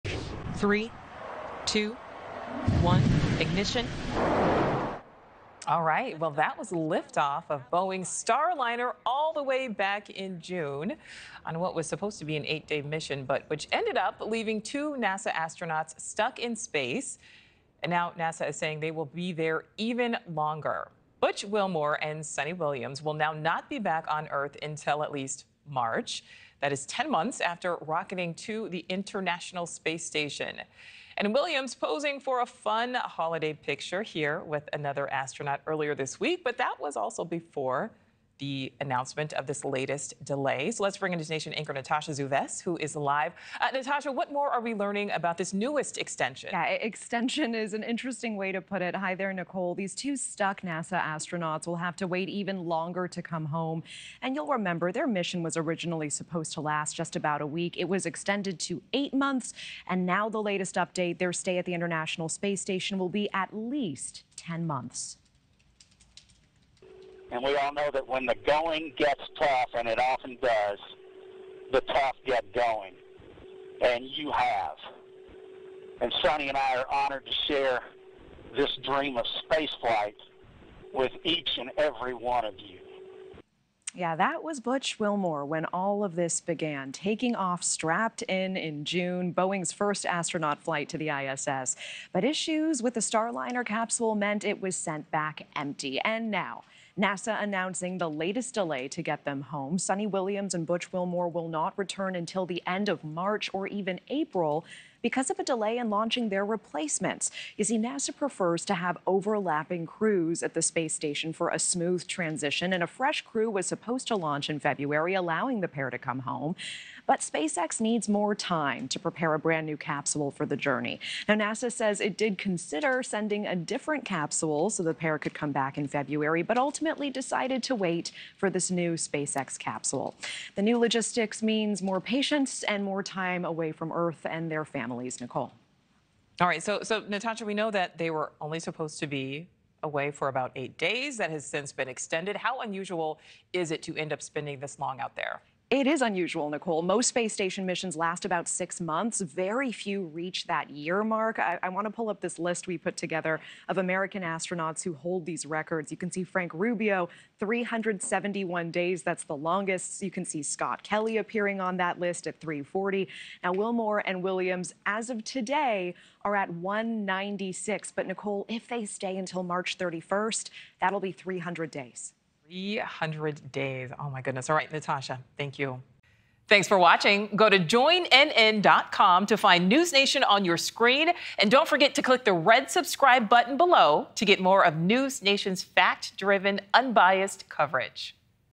NASA's 2 stuck astronauts face more time in space with return delayed until at least late March | Ne
NASA’s two stuck astronauts just got their space mission extended again. That means they won’t be back on Earth until spring, 10 months after rocketing into orbit on Boeing’s Starliner capsule.